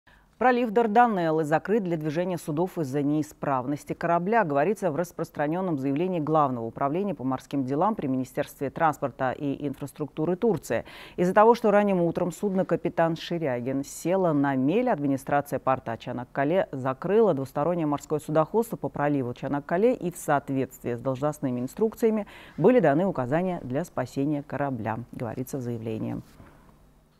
Пролив Дарданеллы закрыт для движения судов
Пролив Дарданеллы закрыт для движения судов из-за неисправности корабля, говорится в распространенном заявлении Главного управления по морских делам при министерстве транспорта и инфраструктуры Турции. «Из-за того, что ранним утром судно «Капитан Ширягин» село на мель, администрация порта Чанаккале закрыла двустороннее морское судоходство по проливу Чанаккале, и в соответствии с должностными инструкциями были даны указания для спасения корабля», — говорится в заявлении.